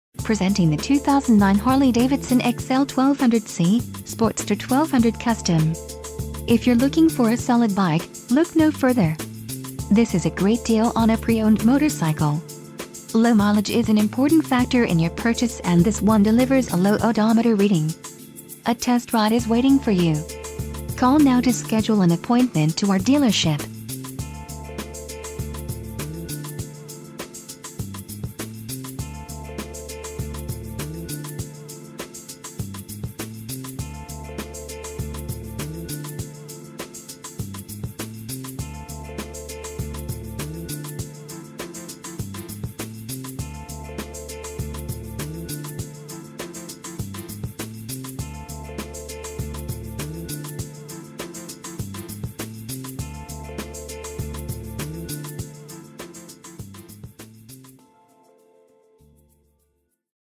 2009 Harley-Davidson® XL1200C - Sportster® 1200 Custom ...
Wow! What a nice Sportster. This super clean XL1200C is nicely done with a set of Vance and Hines exhaust, windshield, and passenger backrest. 2009 Harley-Davidson&#174; Sportster&#174; 1200 CustomPacking plenty of power and just the right mix of chrome, the Sportster&#174; 1200 Custom stays true to its name. With a new lighter-weight chrome Slotted Disc Cast Aluminum rear wheel, chrome Laced Steel 21-inch front wheel with new fender, chrome engine highlights, chrome handlebar and headlight, and a polished fuel tank console, the 1200 Custom shimmers with style. Forward controls and a pull-back handlebar riser give the rider a cool-for-cruising position in the saddle.